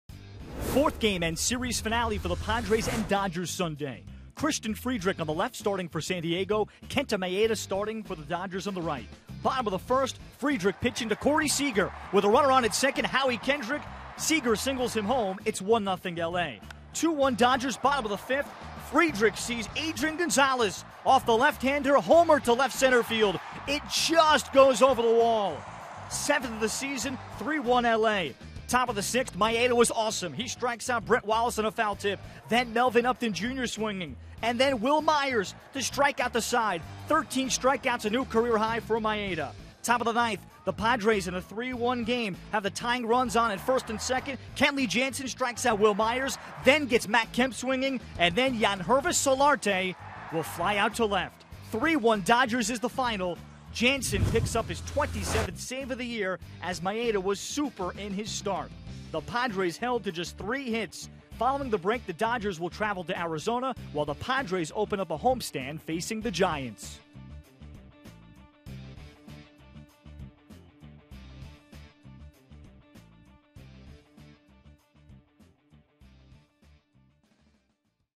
7/10/16: Maeda strikes out 13 to lead LA past Padres
Daily Recap: Adrian Gonzalez homered and Kenta Maeda set a new career-high in strikeouts with 13 in the Dodgers' last game before the break